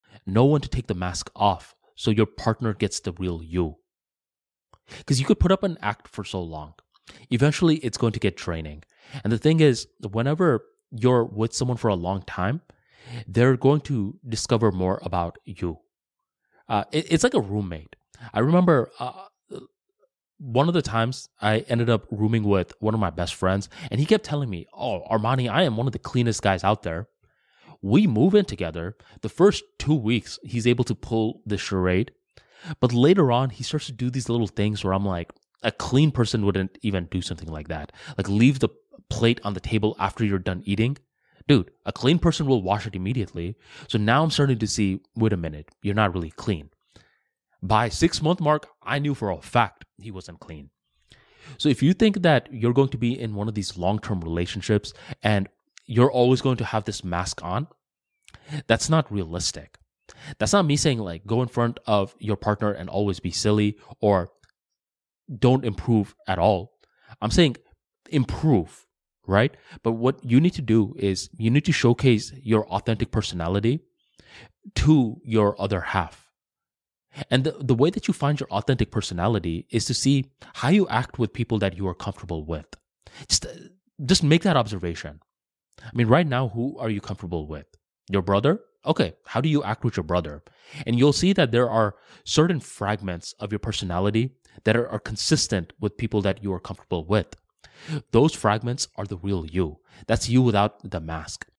How to Be Your Authentic Self in a Relationship
This video shares how to be your authentic self in a relationship. You will learn the basics of carrying yourself without a mask. Learn to identify your authentic personality.

– ArmaniTalks 🎙️🔥